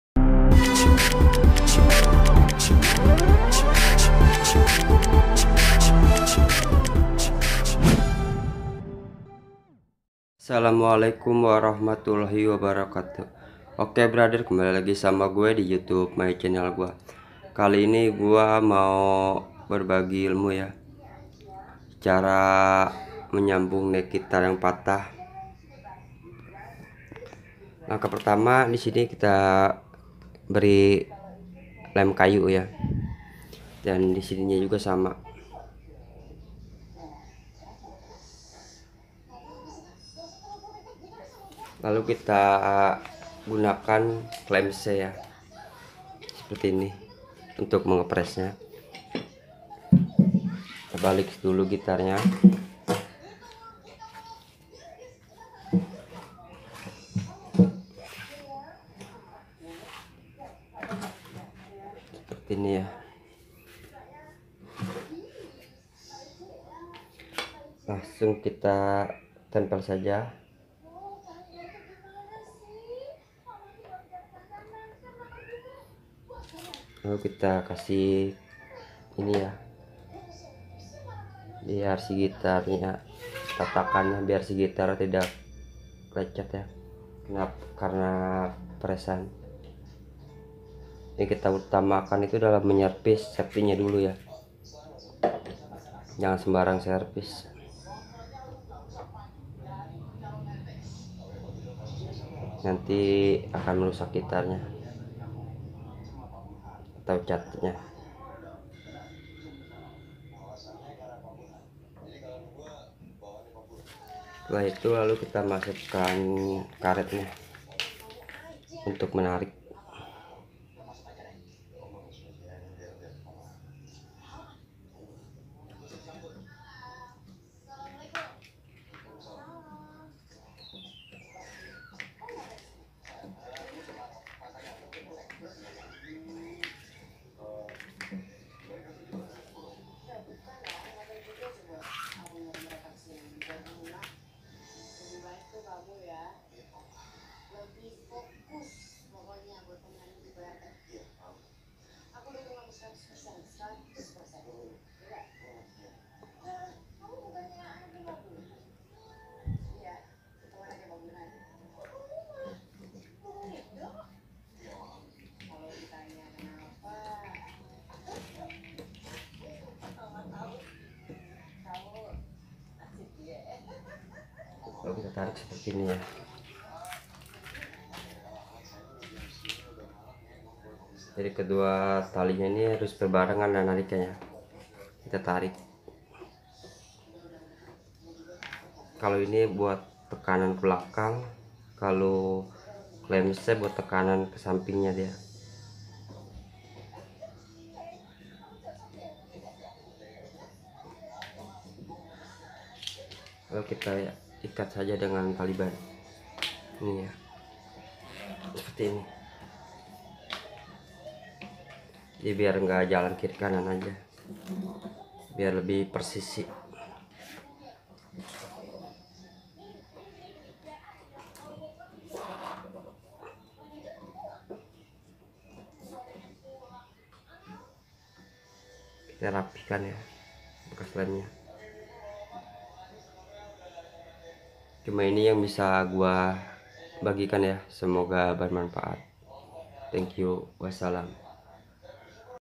CARA MEMPERBAIKI NECK GITAR PATAH
Kali ini saya mau berbagi cara servis neck gitar yang patah dengan cara yang mudah dan dengan alat apa ada nya.